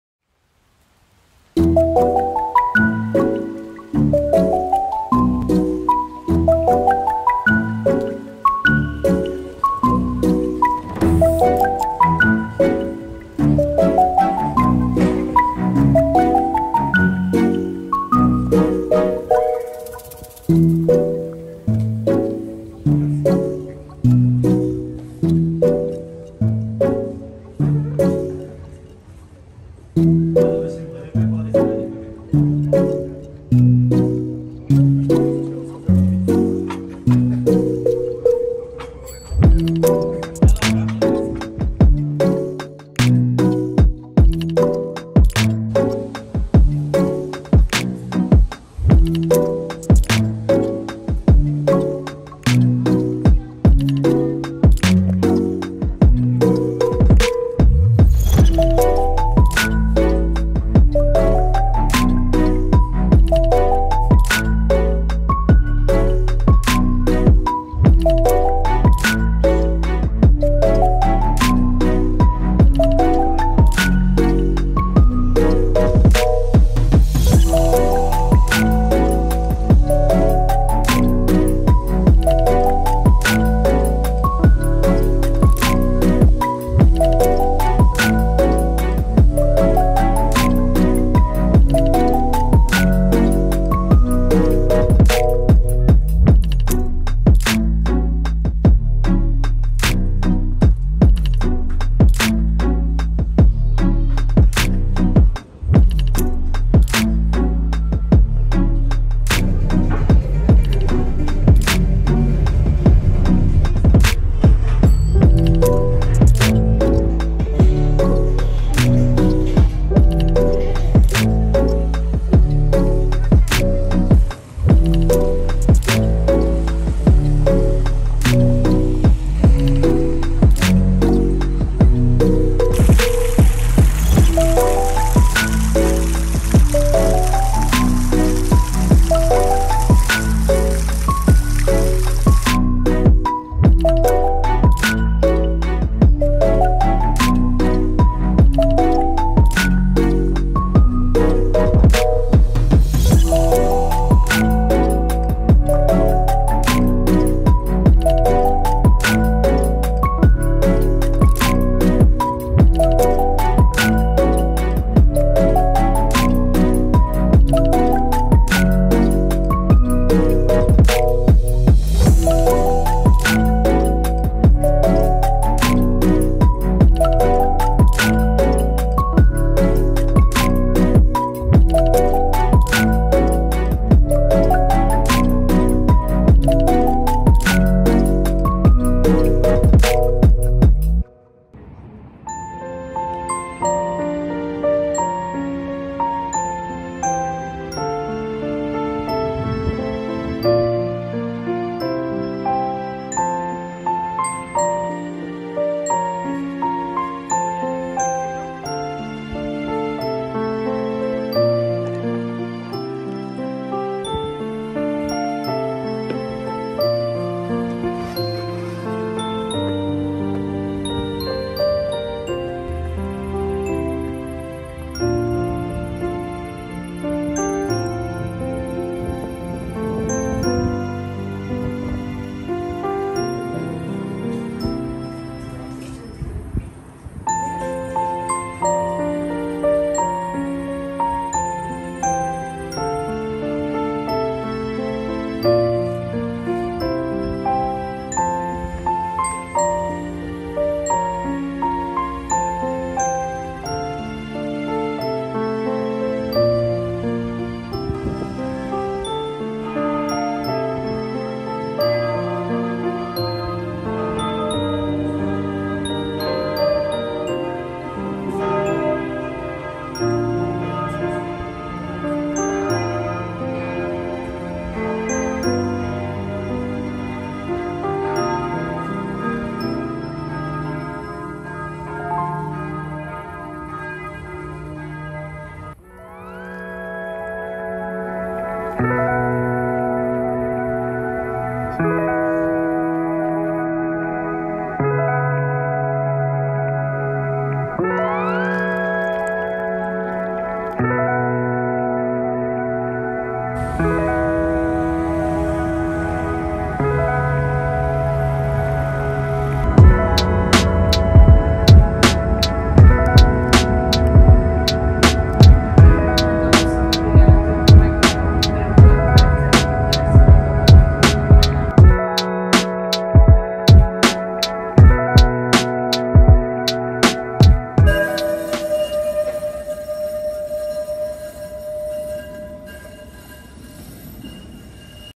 【ハンガリー】ブダペスト漁夫の砦 Fisherman's Bastion
ブダペストはとにかく品のある街。
ブダ側にある漁夫の砦から美しい街並みを眺める。
夕方〜夜景をみる最高の場所。